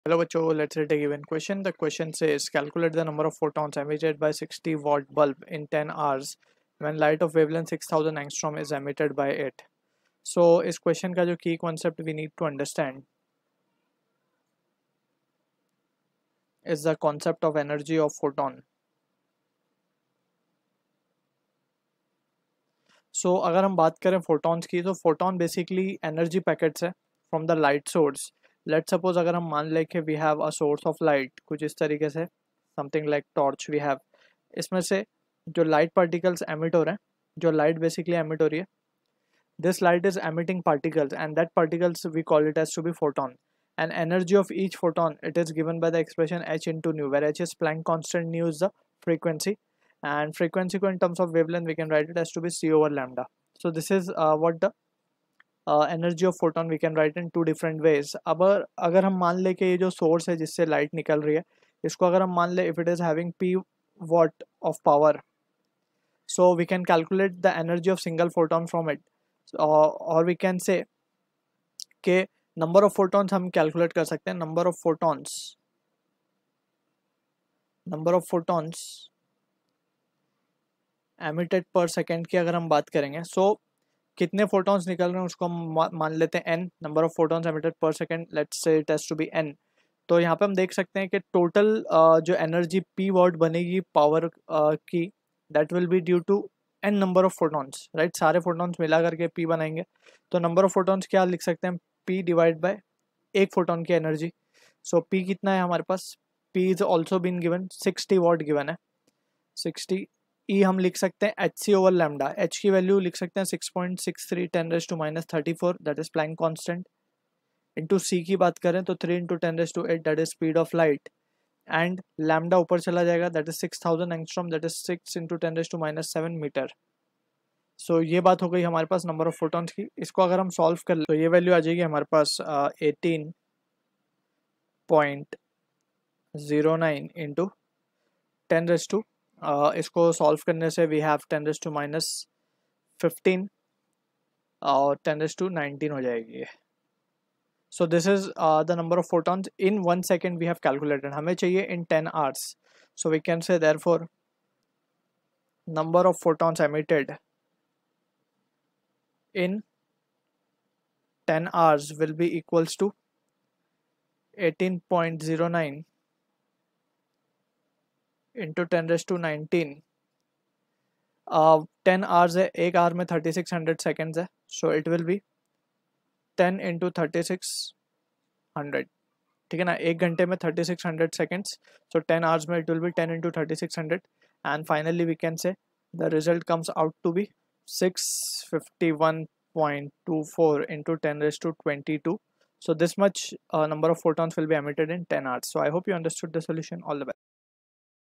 Calculate the no. of photons emitted by 60 watt bulb.in 10 hrs. When light of wavelength \( 6000...
Calculate the no. of photons emitted by 60 watt bulb.in 10 hrs. When light of wavelength \( 6000 \AA \) is emitted by it.